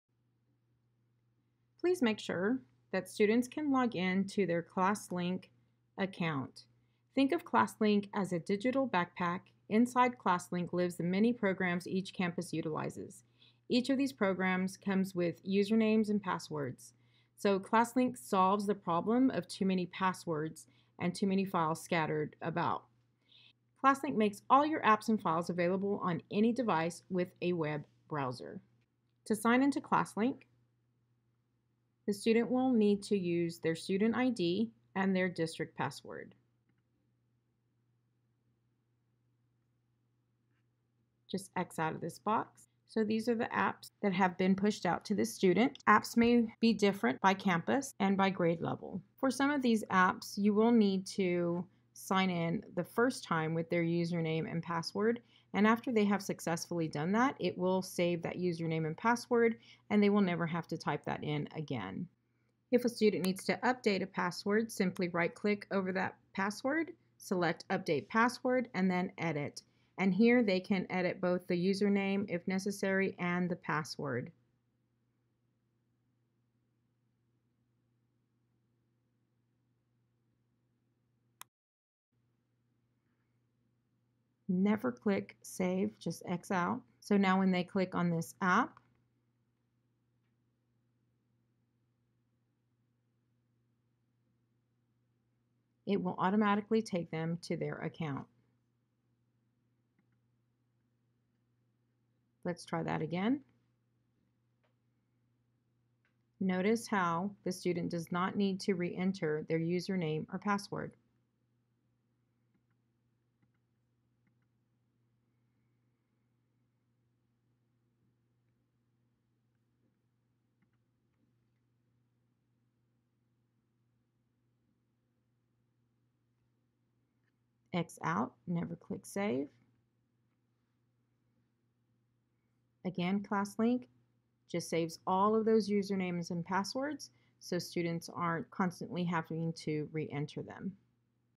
Accessing Classlink
What is Classlink and how do students use it?